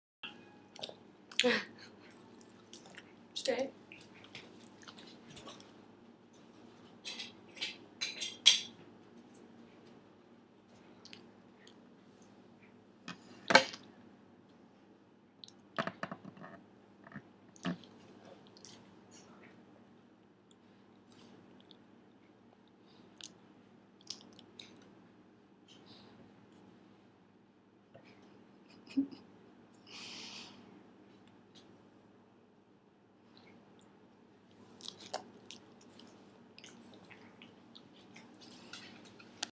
Dog Turns Into Cat
Miles the Cavalier King Charles Spaniel turns into a cat with filter. 2011